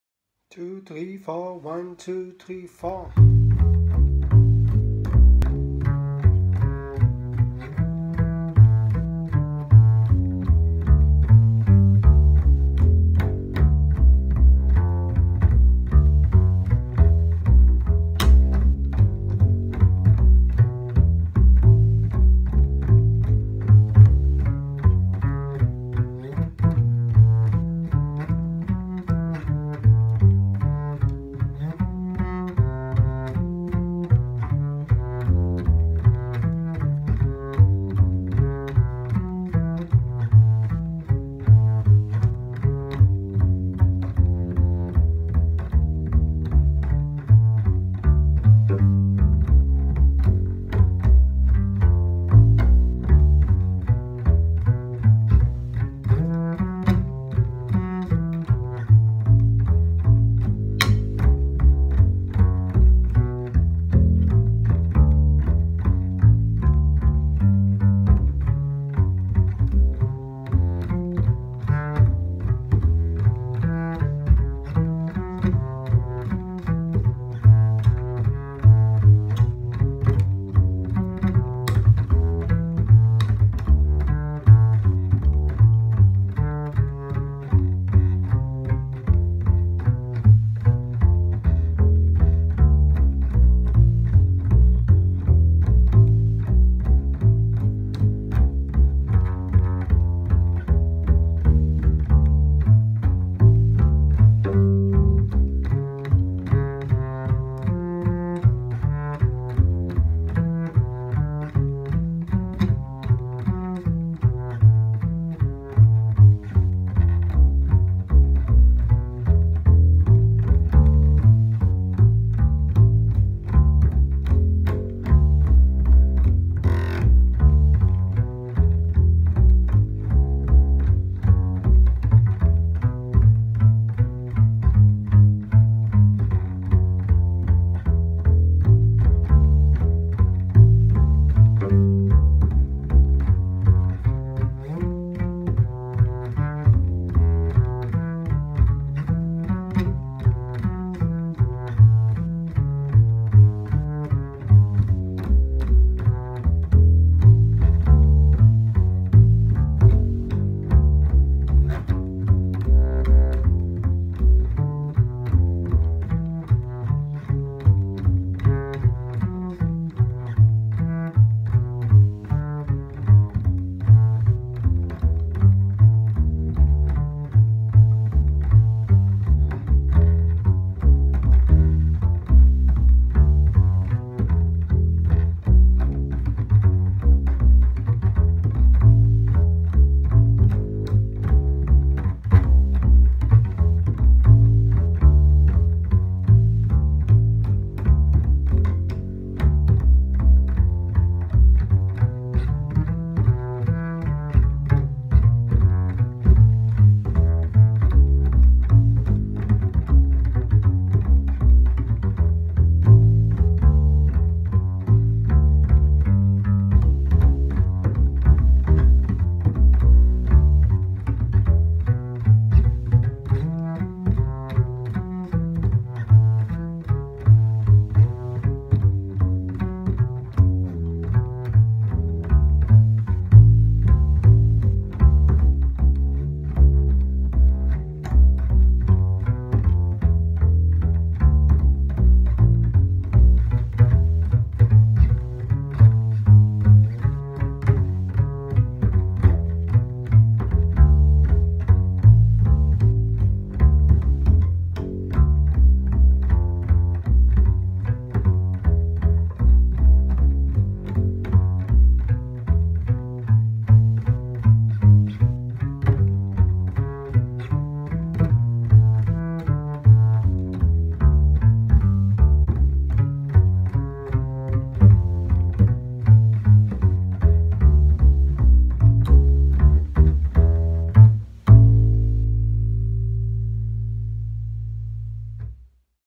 Bird Blues in A Bass Line Bass Line Backing Track
Bird Blues in A Bass Line Play Alone Backing Track
In A major, 15 chorus, 155 bpm.